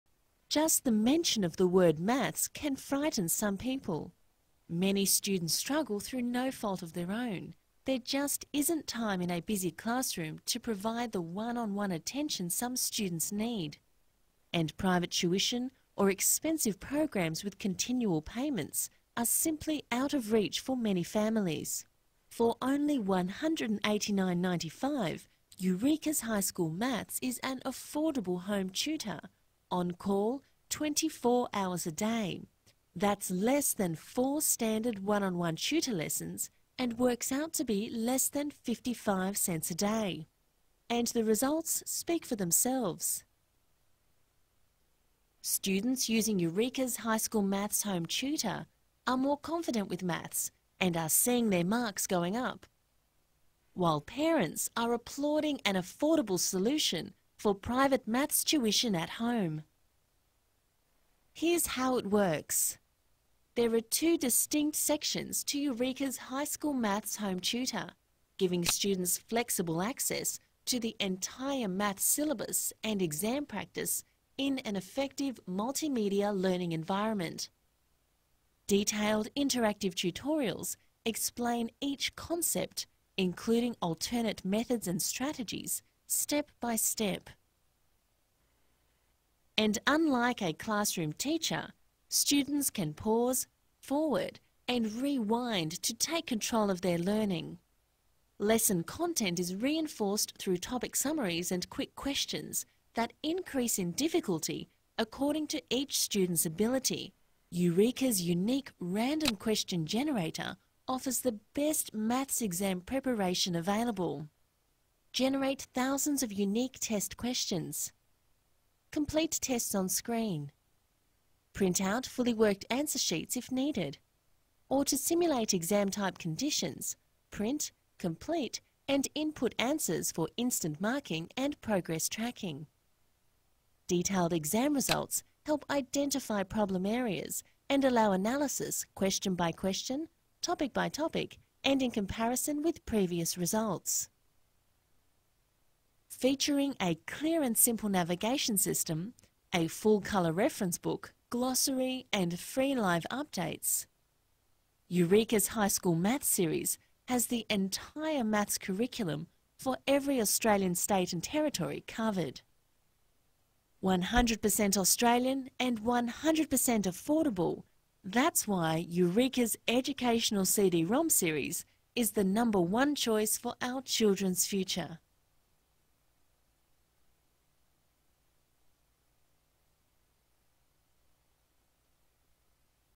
Eureka's High School Maths Home Tutor
Eureka's High School Maths Home Tutor is like having a tutor at home 24/7, at a fraction of the cost.

   - Recommended by over 90% of parents and teachers.
   - Improves maths results by up to 50%.
   - Complete and comprehensive.

Features

   - Fully interactive animated 'Talking Tutor' with pause and rewind controls;
   - Millions of randomly generated syllabus style practice questions;
   - Detailed step-by-step worked solutions;
   - Covers the entire year's syllabus;
   - Automatic storing and graphical display of test results;
   - Free updates using Eureka's Live Updater;
   - Full colour reference book (over 200 pages); and
   - Full colour 48-page user manual.